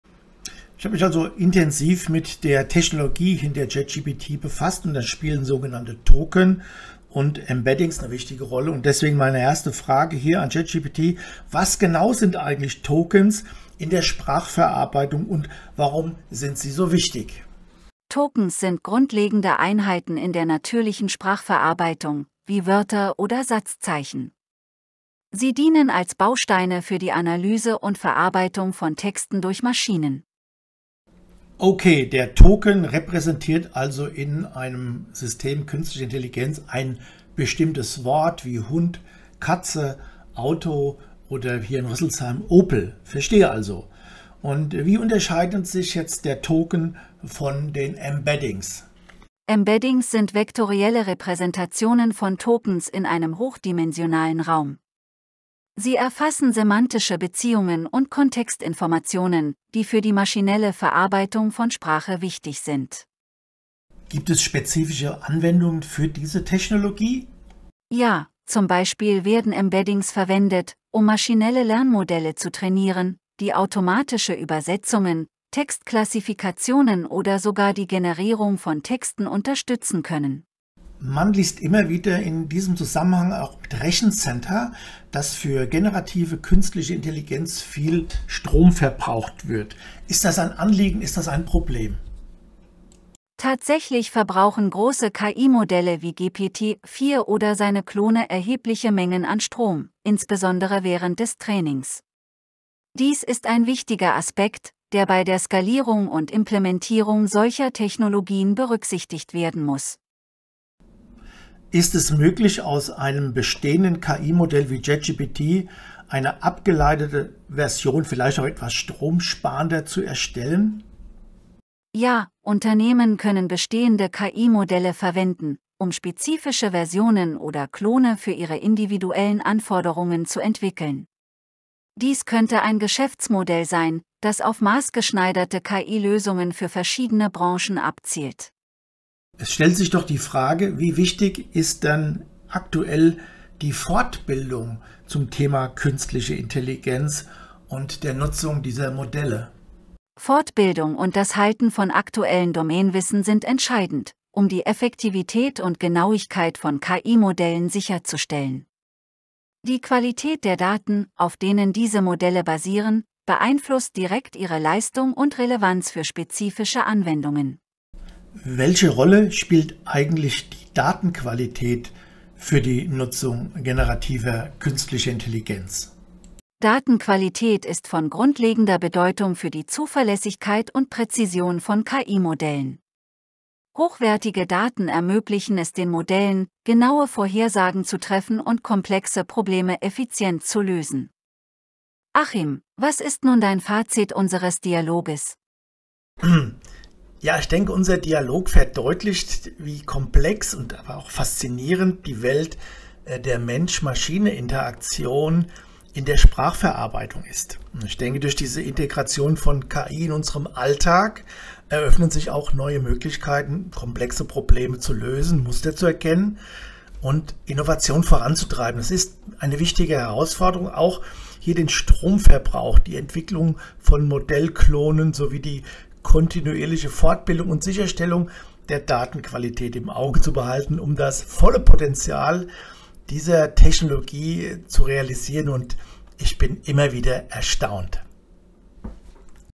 Über Token, Embeddings und Stromverbrauch: Ein Nachmittag mit ChatGPT
Zurzeit arbeite ich an einem neuen Modul: "KI-Grundlagen und Tools für die Unternehmerpraxis". Die Nutzung generativer KI ist mehr als nur "googeln". Sie erfordert die Dialogfähigkeit des Nutzers vor dem Bildschirm oder am Smartphone. Wer sich für die Technik hinter dem Eingabeschlitz für den Prompt interessiert, kommt an den technischen Details nicht vorbei. Bei meinem Dialog am Nachmittag kam eine Menge Stoff zusammen. Alles ist in diesem Podcast zusammengefasst.